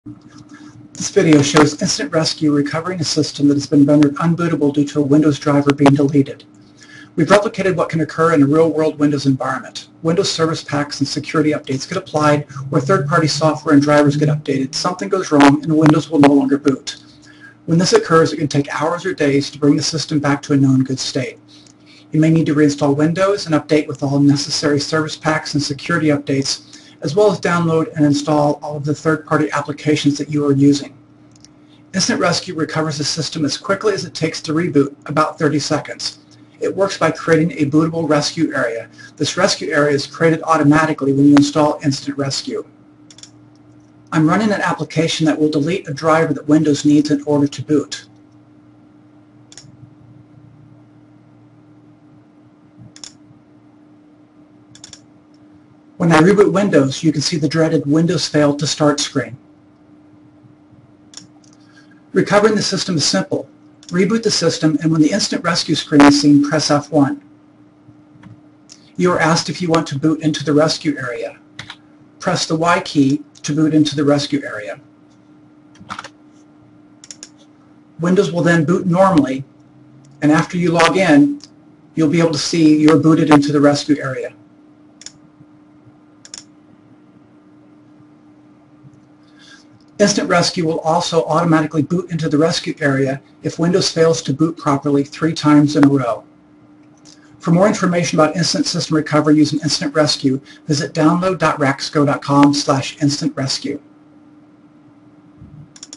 Live Demo: Restore a Windows PC That Can't Boot with InstantRescue
What do you do when your system can't boot? If you have InstantRescue installed, you can avoid reinstalling Windows and all the third-party applications you had running on your computer. Just simply boot into your InstantRescue Rescue Area after your system becomes unbootable and your system will react as if the crash never occurred in the first place.